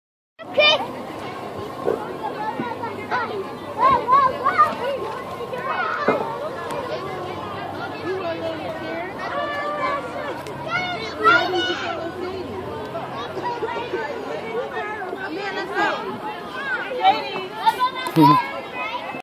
Halloween at La Plaza 2
A Halloween celebration at La Plaza Community Garden in the Lower East Side of Manhattan (2017)